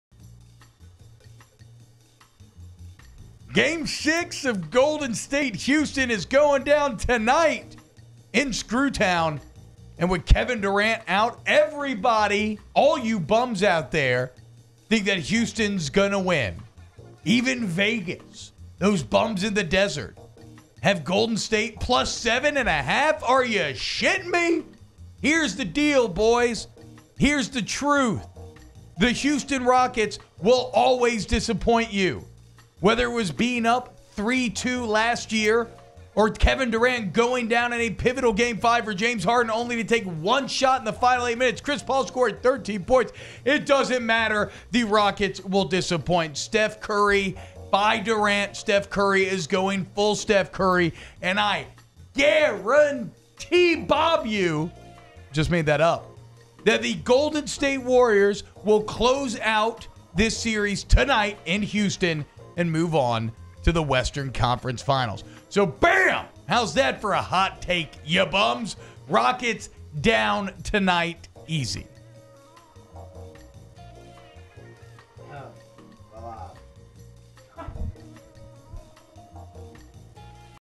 Why Houston is Gonna Lose To The Warriors
OFF THE BENCH HOT TAKES
Houston always disappoints! T-Bob breaks down his hot take of why Golden State is gonna beat out the Rockets even though they don't have Durant.